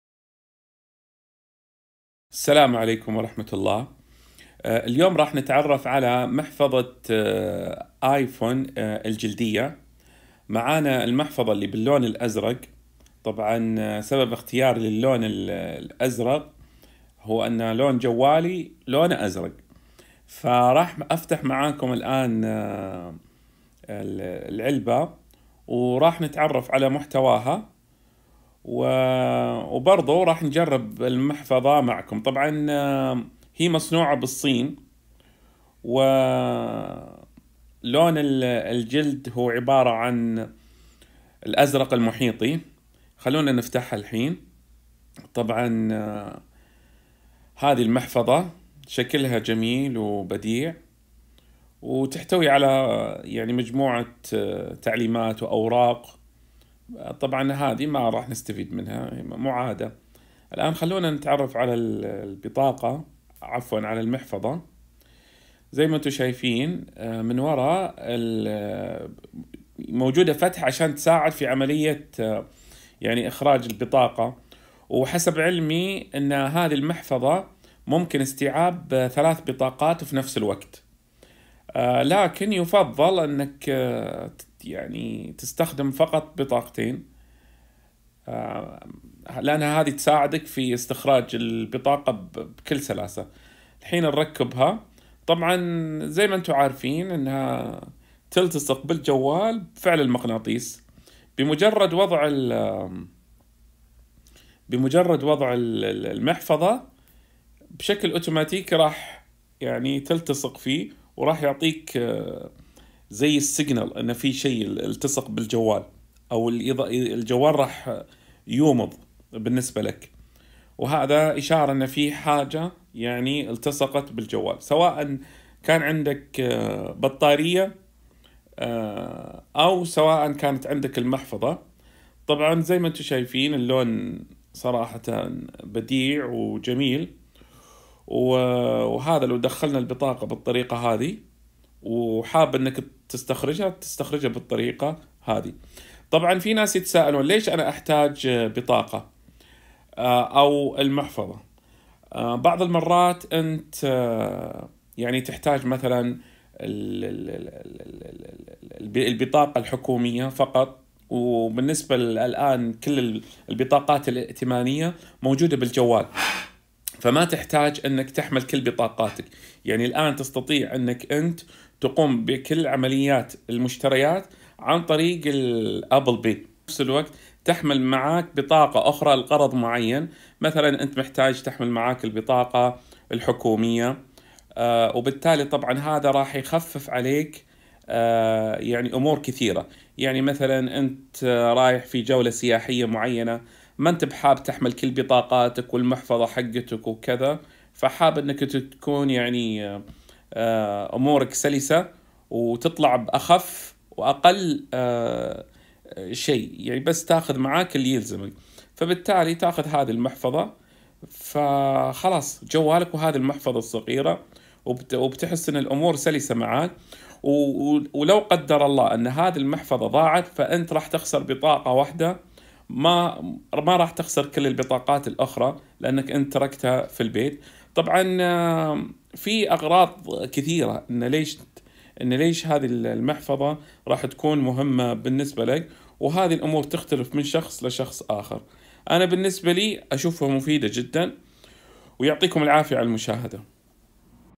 محفظة ايفون ١٢ ماكس
محفظة ايفون الجلديه الخاصه بجوال ايفون ١٢ ماكس
اللون الازرق المحيطي